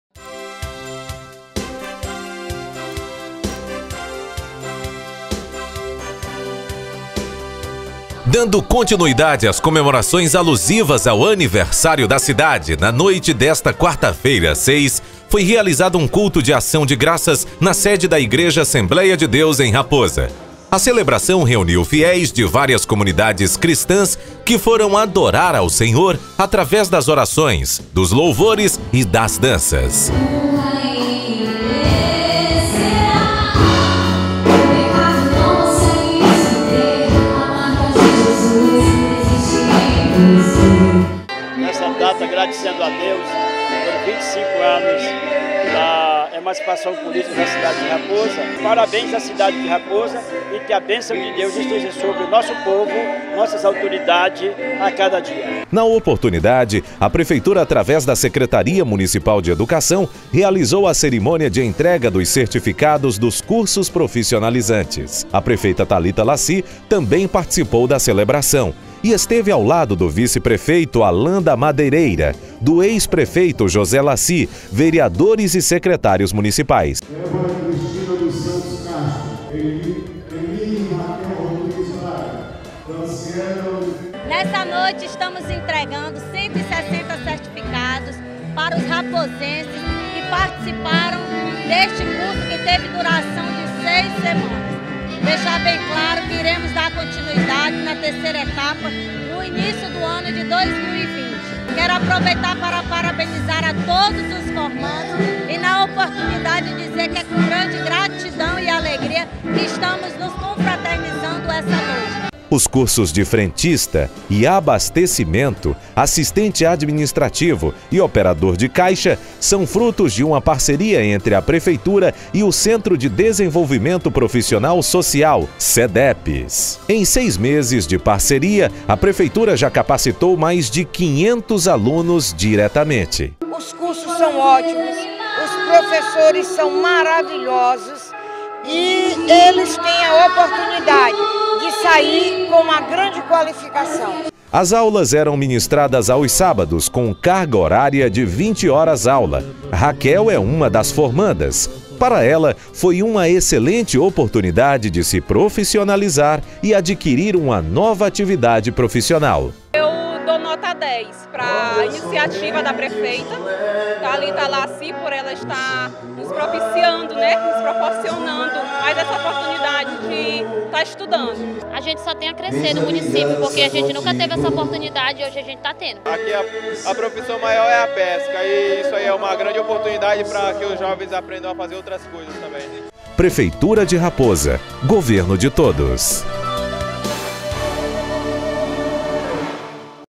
RAPOSA - ENTREGA CERTIFICADOS DOS CURSOS PROFISSIONALIZANTES
Promover desenvolvimento por meio da capacitação. Na noite desta quarta-feira (6), a Prefeitura Municipal de Raposa, através da Secretaria Municipal de Educação, realizou a cerimônia de entrega dos certificados dos cursos profissionalizantes. O evento aconteceu na sede da igreja Assembleia de Deus. São raposenses que agora estão mais preparados para o mercado de trabalho.

Veja o vídeo: